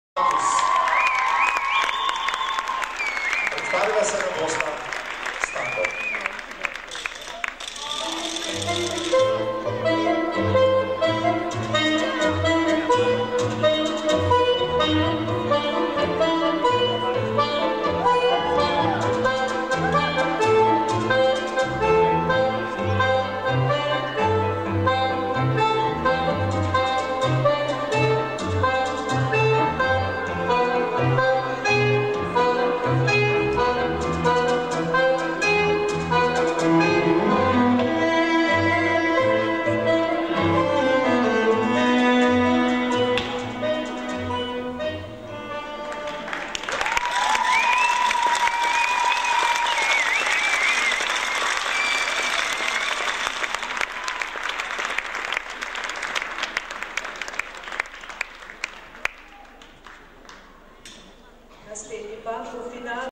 Miša Cigoj & Anna Matus Presentation Dance Tango
National Standard Championship 2009